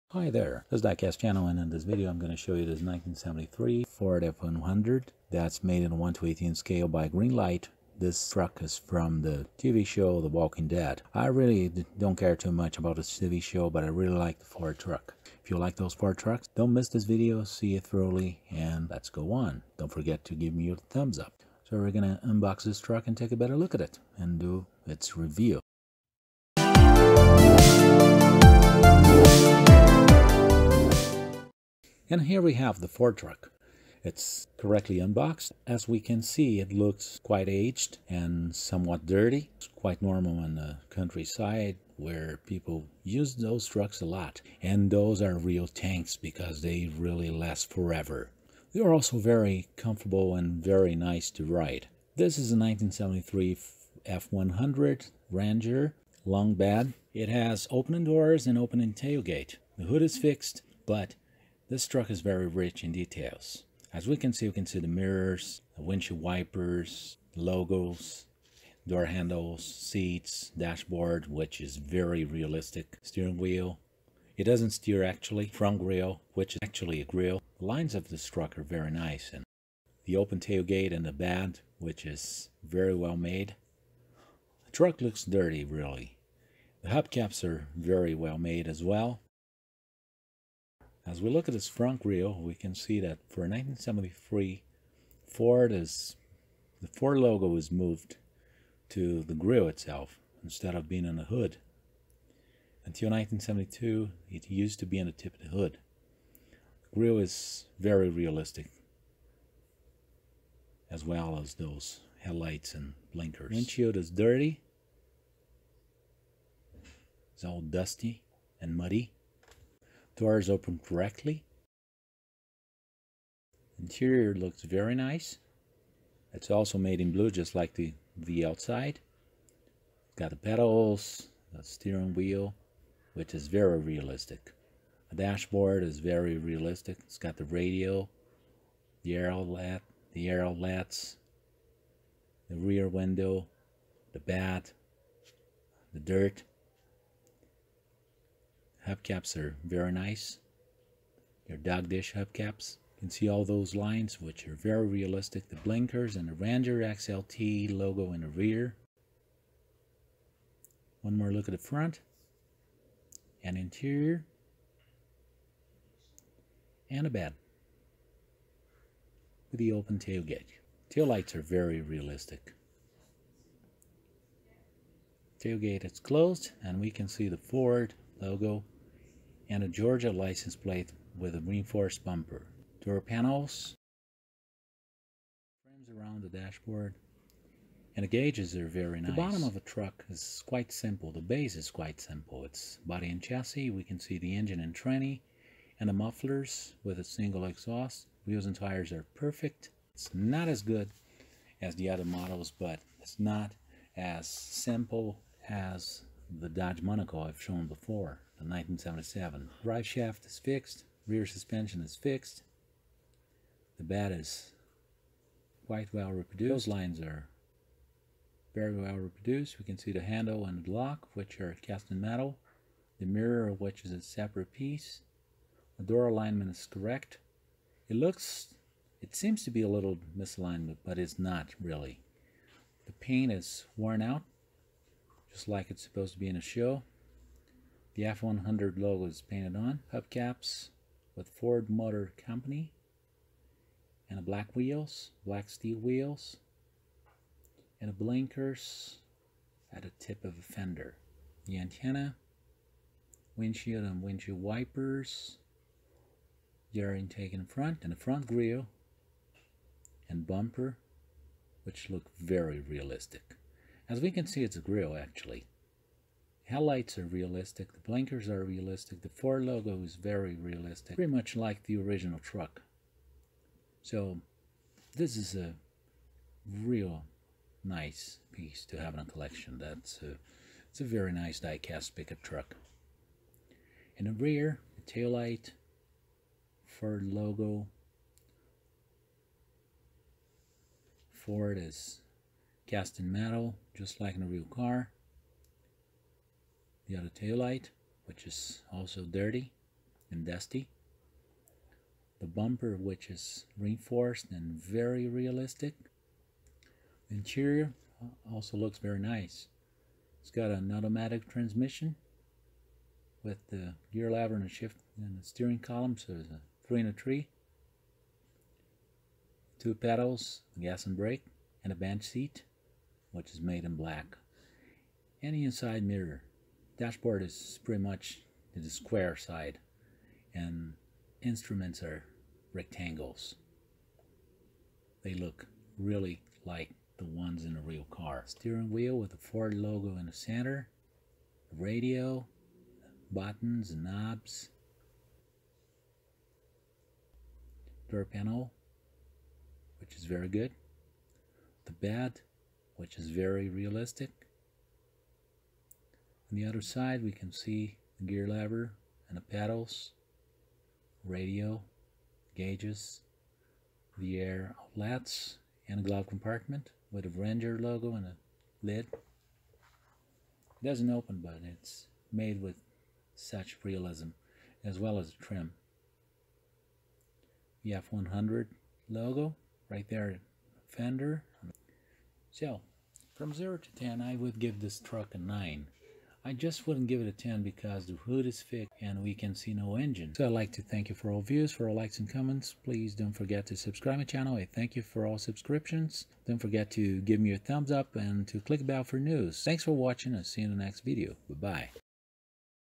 Review of a 1:18 1973 Ford F 100 from "The Walking Dead", by Greenlight
I've always liked Ford trucks a lot, and this '73 F 100 is no exception!
Although I have a preference for pristine looking cars, this "worn out and dirty" Ford pick-up has really caught my eyes!